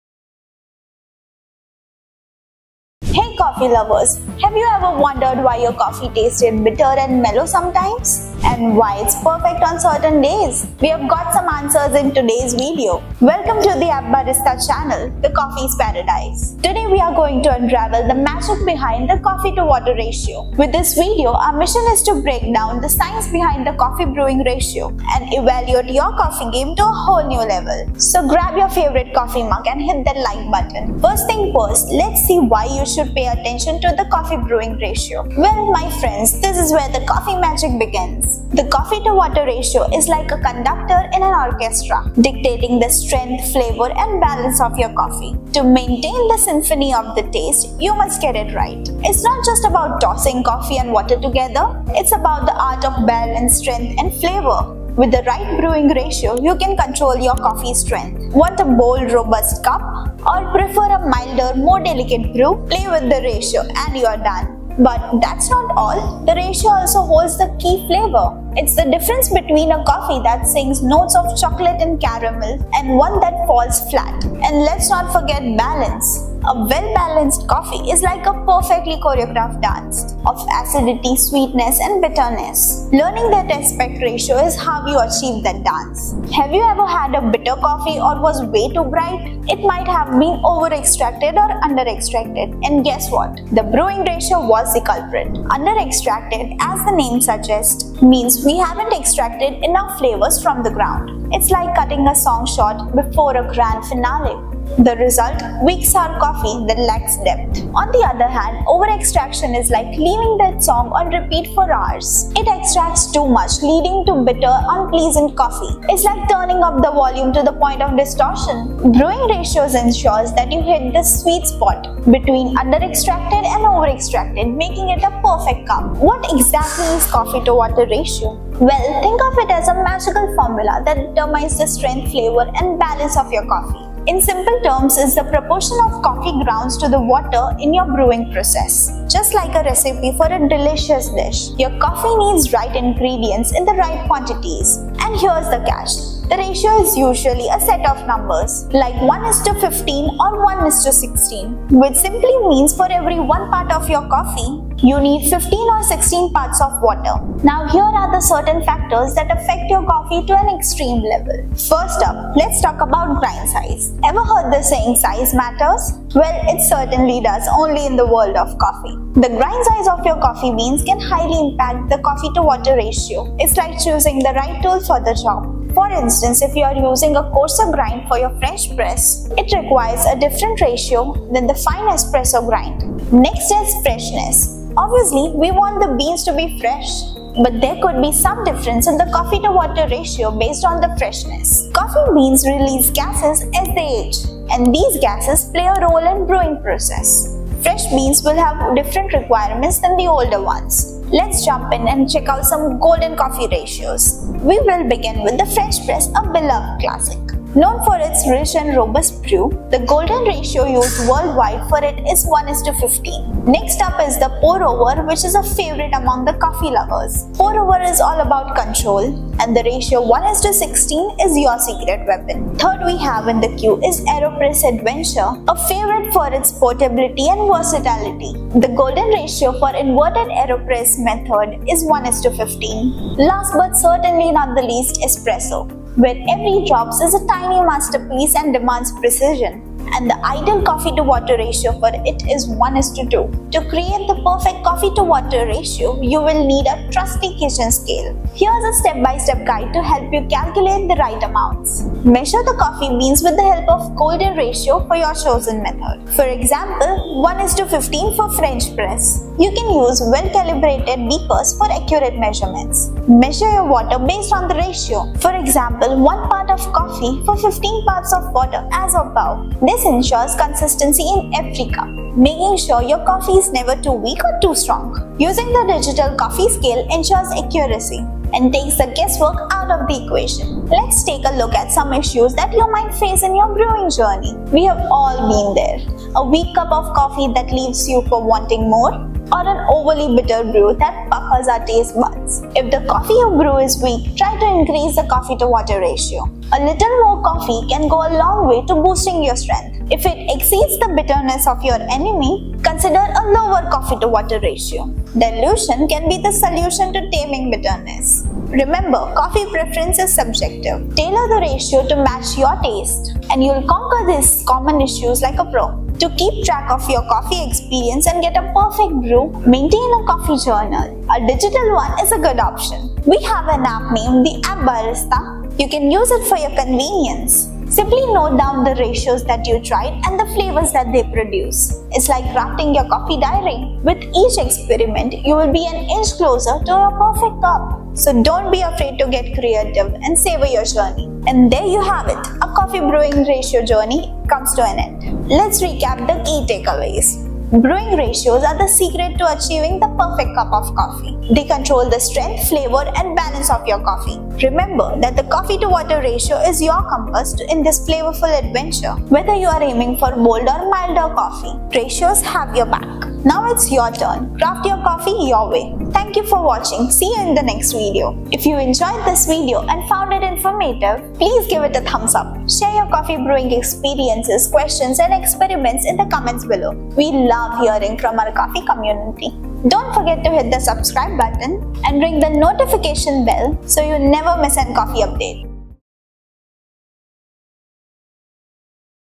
Coffee Brewing Ratios Explained | Use These Brew Ratios to Brew Better Coffee
In this video, we will introduce you with different coffee to water ratios. We hope having idea about brewing ratios will help you brew a better cup of coffee everytime.

Along with different ratios, we have also include golden coffee brewing ratios for different coffee methods like french press, pour-over, aeropress, etc.